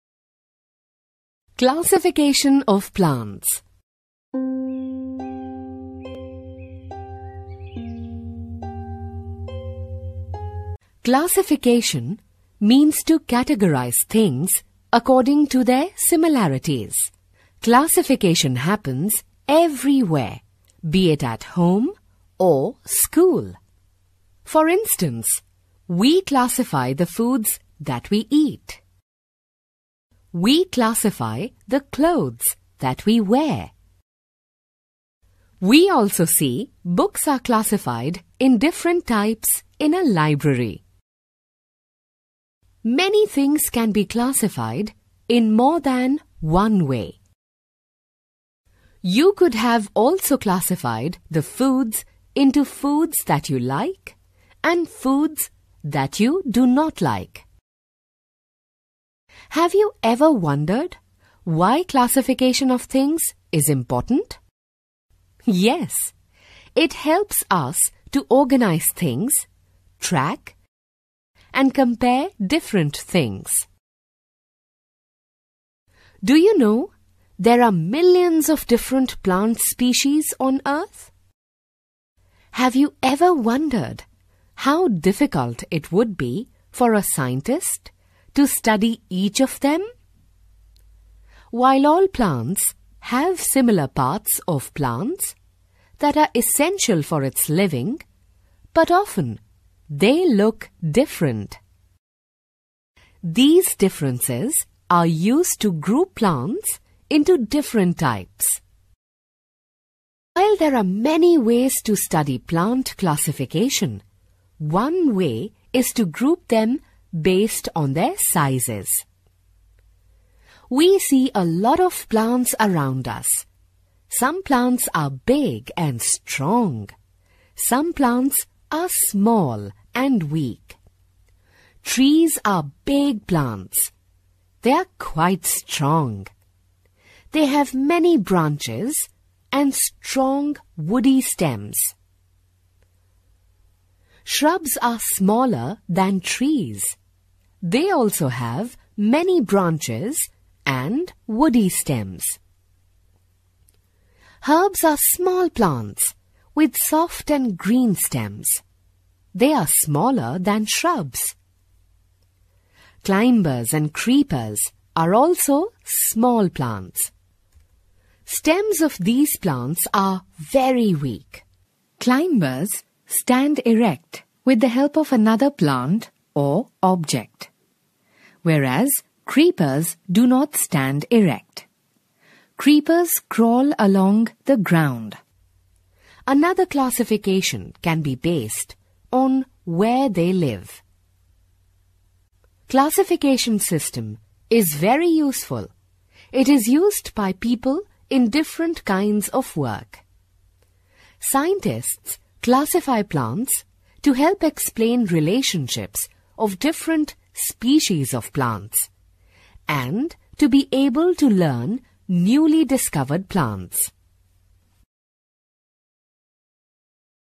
Classification of Plants | Macmillan Education India
Watch this animation to understand the importance of classification in our lives and learn about how plants are classified into different groups.